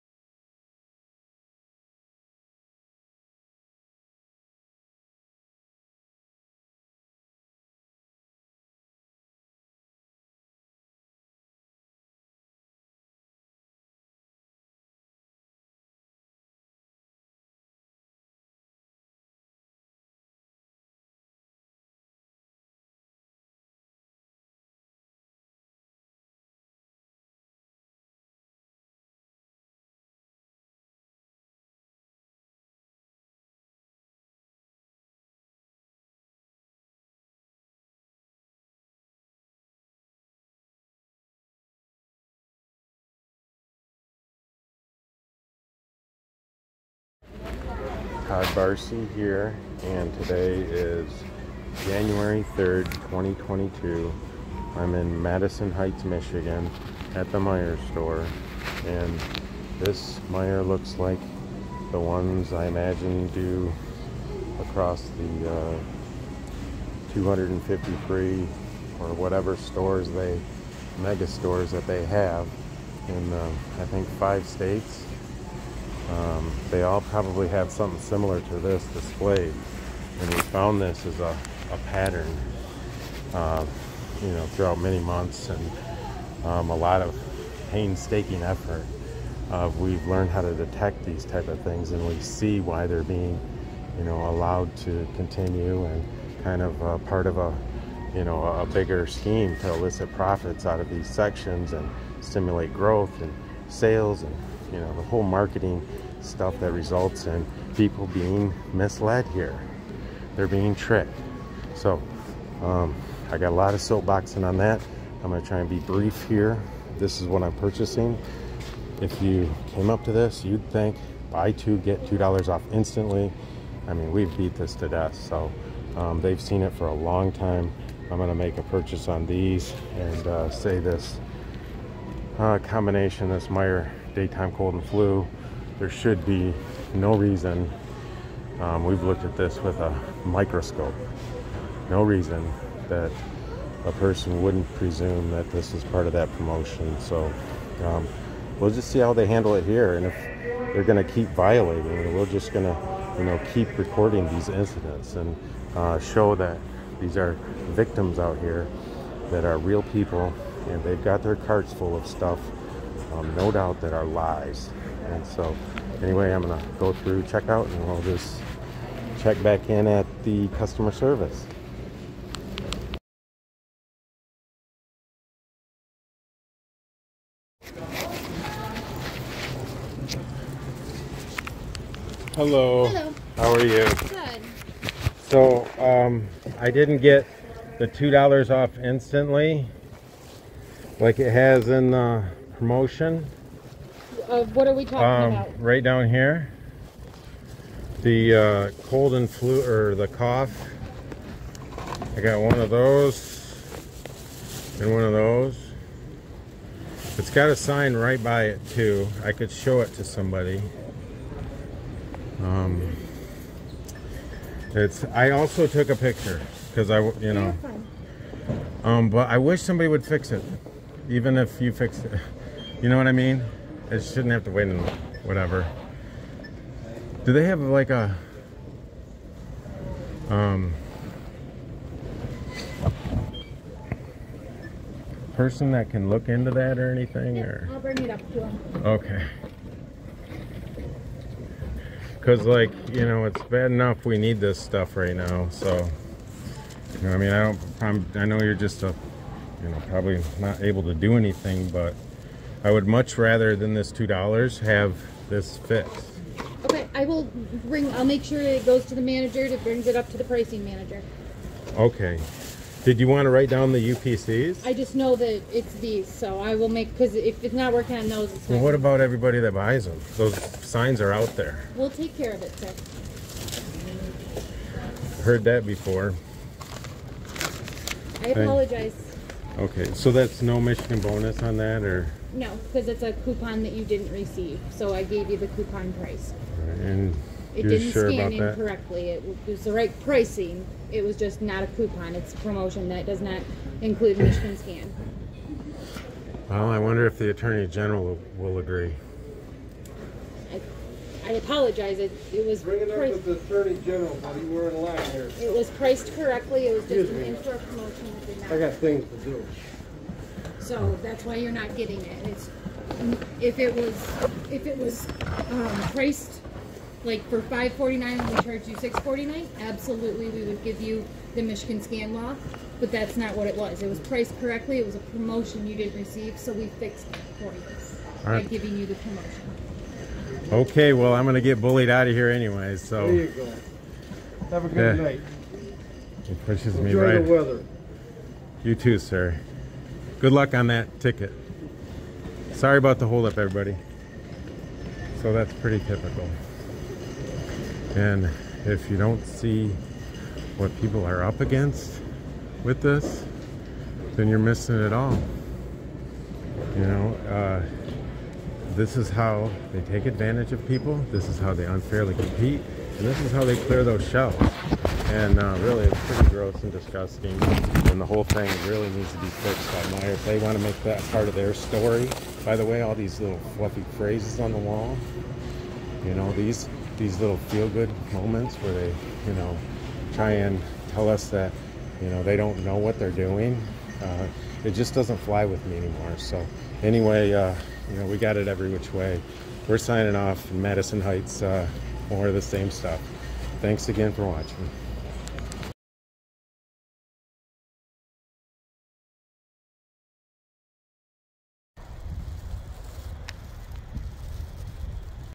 Jan 3 2022 Meijer line & bad policy SHOO AWAY victims. Cough & Cold NEVER FIXED fake sale continues
Madison Hts, MI 1-3-22 See how this system feeds itself with non-compliance with our state's consumer protection laws that afford Meijer opportunities to bar the violations from prosecution. They are clearly choosing not to do that by matter of policy, impracticality--as you can see this impatient lottery player joins in on pushing us out.